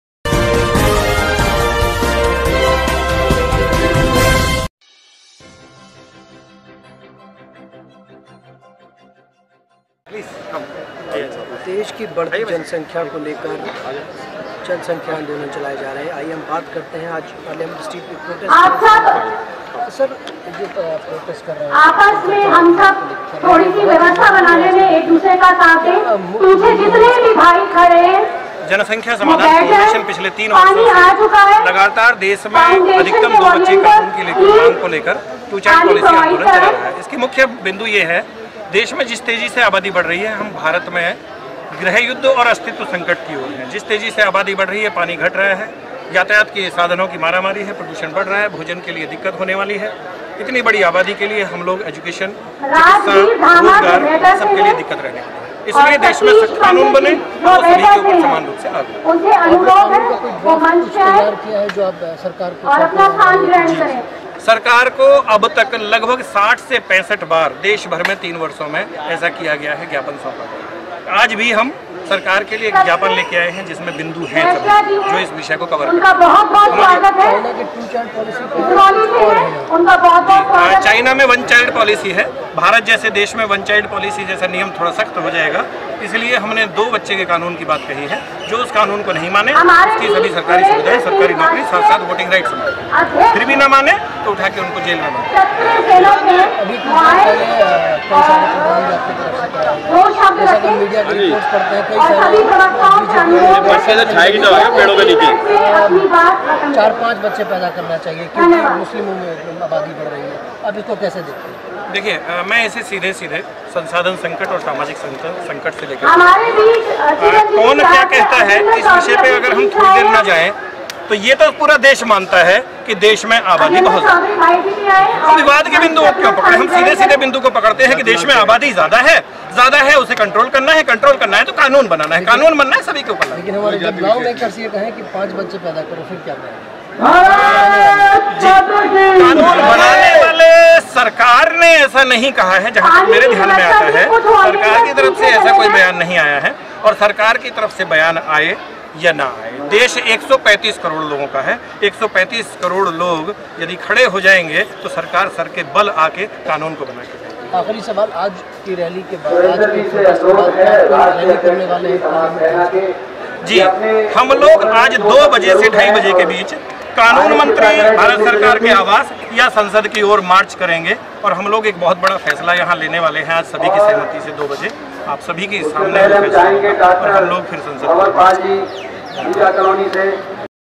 NDA govt. should pass Two Child Policy Bill in Monsoon season of Parliament | Anil Choudhary
NDA govt. should propose Two Child Policy Bill in Monsoon season of Parliament | Anil Choudhary